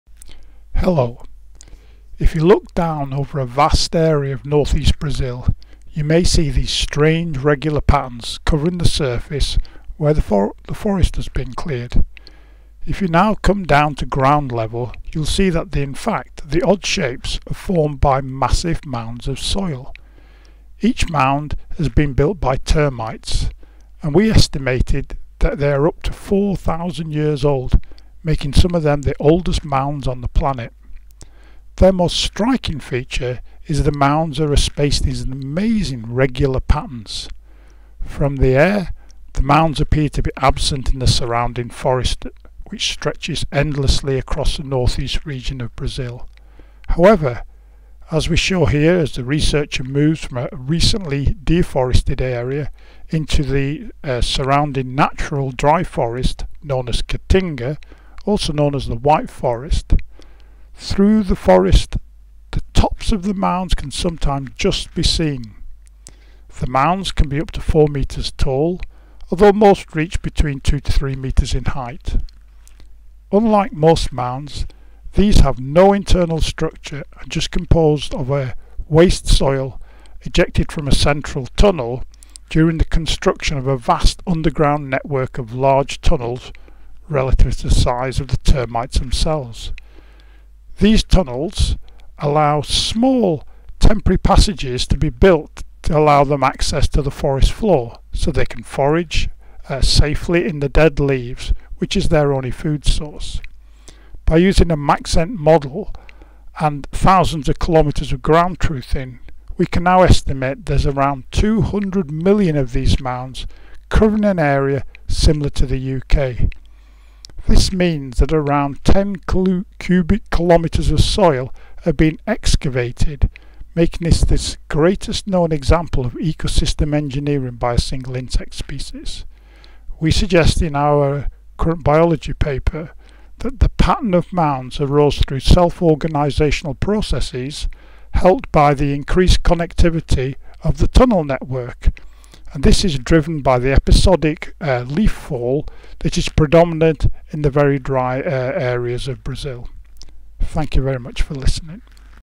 Vast 4,000-Year-Old Spatial Pattern of Termite Mounds/Curr. Biol., Nov. 19, 2018 (Vol. 28, Issue 22)
A vast array of 200 million termite mounds have existed in NE Brazil for up to 4,000 years. These mounds cover 230,000 km2, an area similar to that of the UK, and are the result of the excavation of 10 km3 of soil during the construction of a massive underground network of tunnels. A combination of time and interconnectivity of the tunnels has allowed a regular (over-dispersed) pattern of mounds to emerge through self-organizational processes driven by episodic leaf fall.

S.J. Martin, R.R. Funch, P.R. Hanson, and E.-H.E. Yoo (2018). Vast 4000-year-old termite population persists in Northeast Brazil. Curr. Biol. 28.